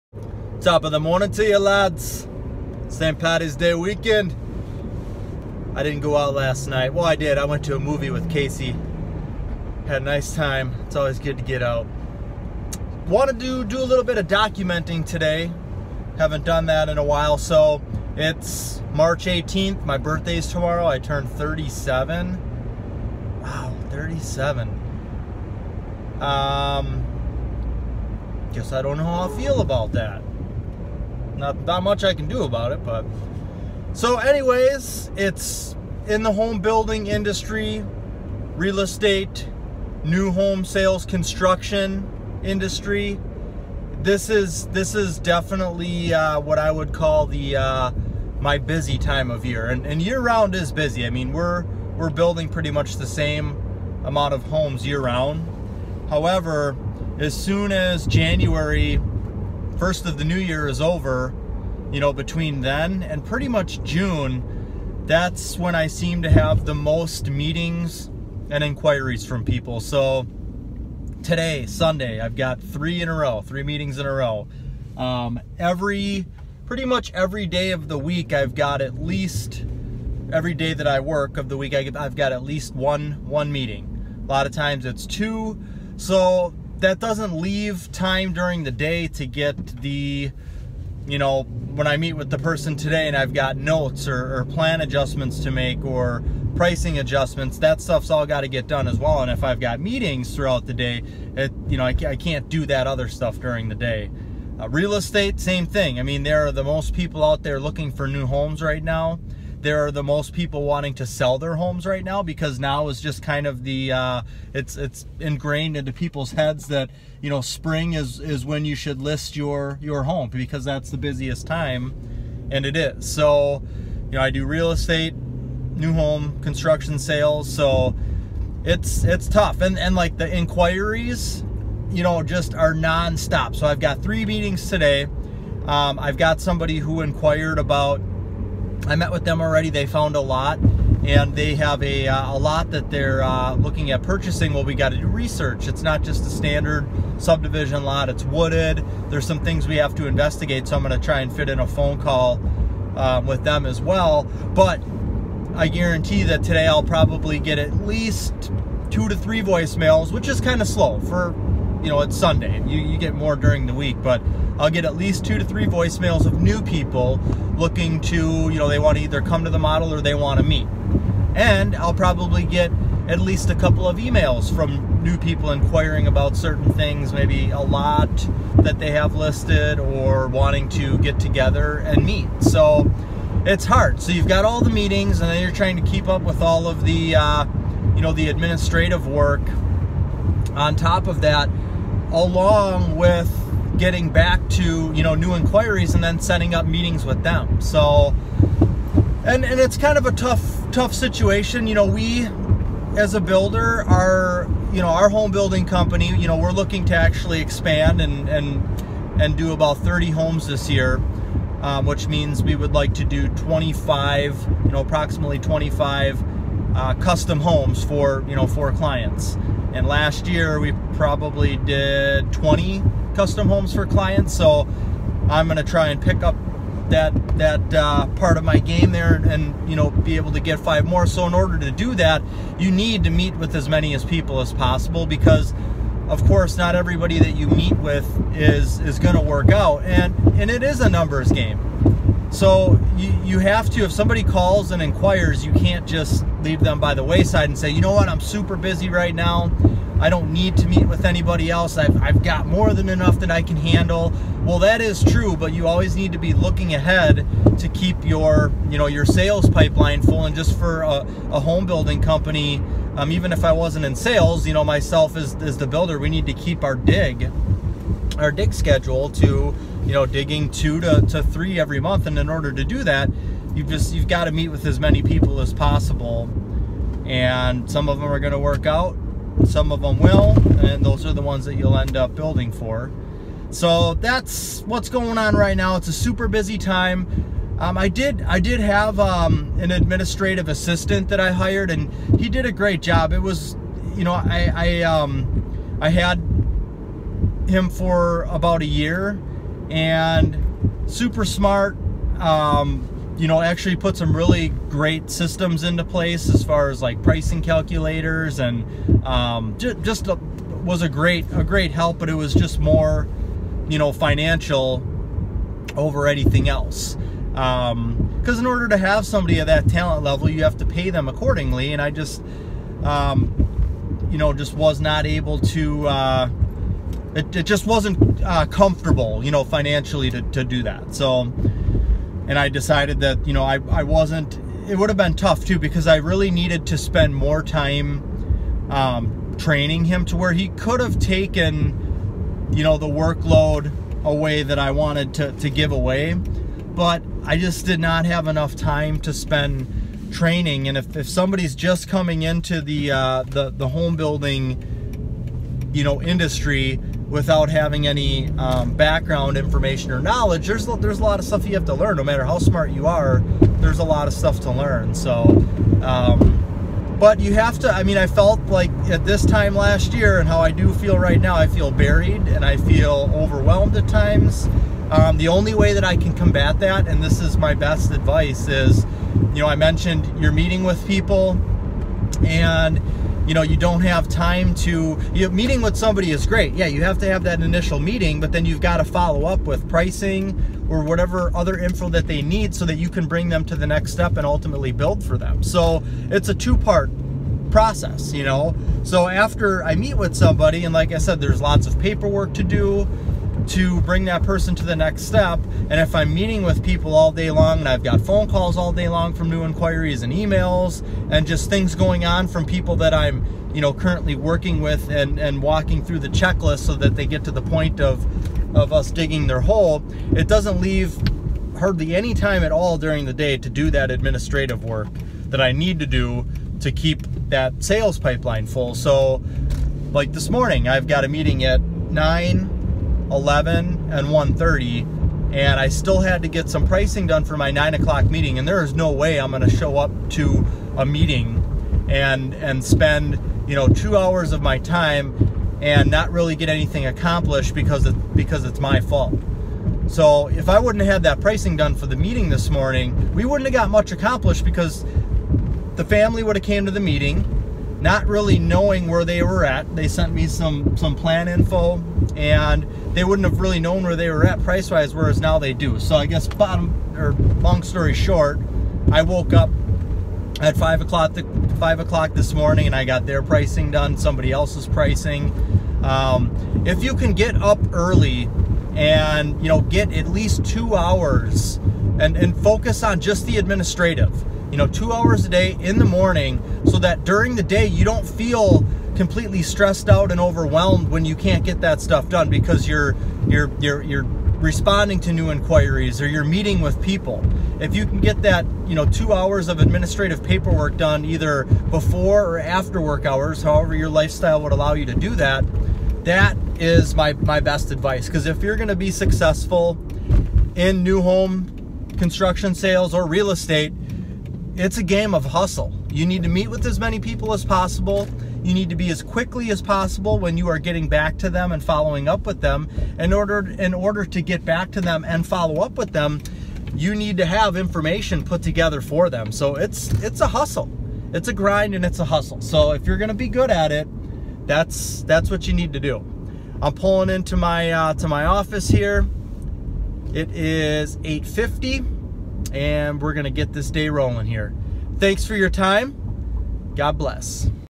Selling New Construction Homes | Documenting my Career
Selling New Construction Homes and documenting my career. I sell new construction homes and Real Estate. I have been documenting this my Real Estate Vlog for almost a year. In this video I explain how things are going today, 3-18-18